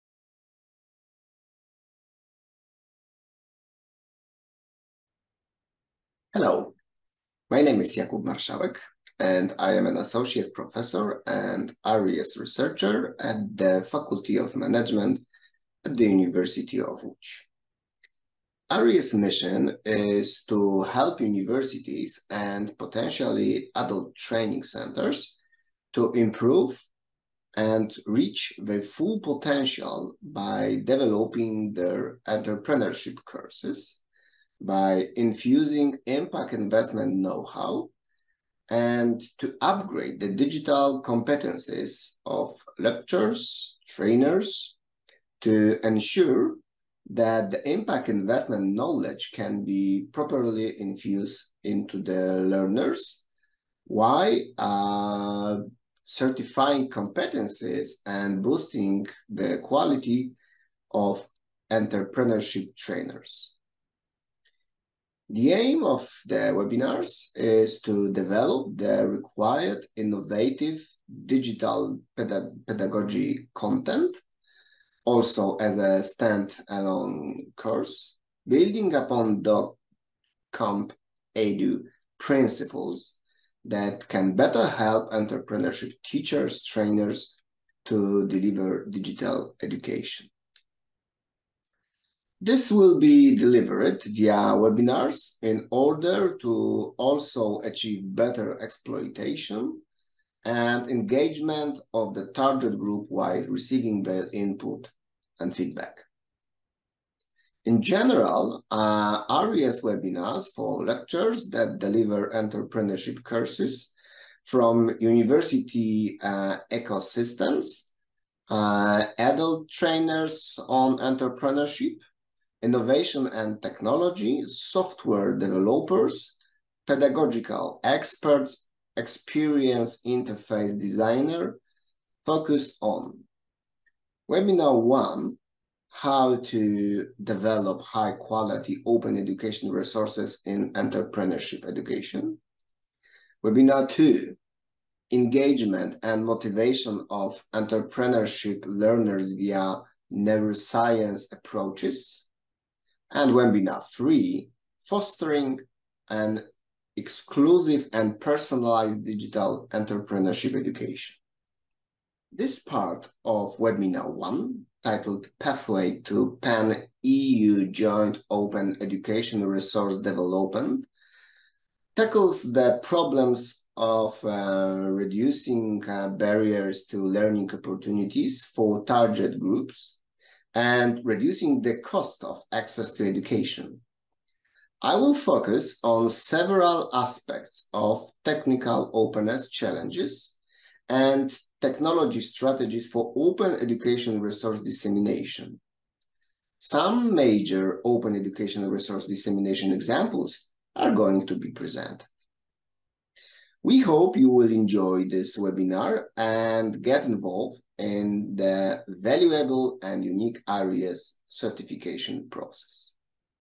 ARIEES TRAIN THE TRAINERS WEBINAR 1 – INTRO How to develop high quality OERs in entrepreneurship
Are you an educator looking to enhance your entrepreneurship curriculum with high-quality Open Educational Resources (OERs)? Look no further than ARIEES!
Our Train the Trainers webinar series is designed to equip educators like you with the tools and knowledge needed to create exceptional OERs in entrepreneurship education. In this first webinar, we'll be focusing on how to develop high-quality OERs from scratch. Led by industry experts, this webinar will provide you with insights and strategies to create engaging, effective, and pedagogically-sound OERs that will help your students thrive in the entrepreneurship field. You'll learn about the latest trends in entrepreneurship education and best practices for developing OERs that meet your students' needs.
Our goal is to help you create OERs that are not only effective but also accessible to a wider range of students. Join and start developing OERs that will inspire and engage your students today!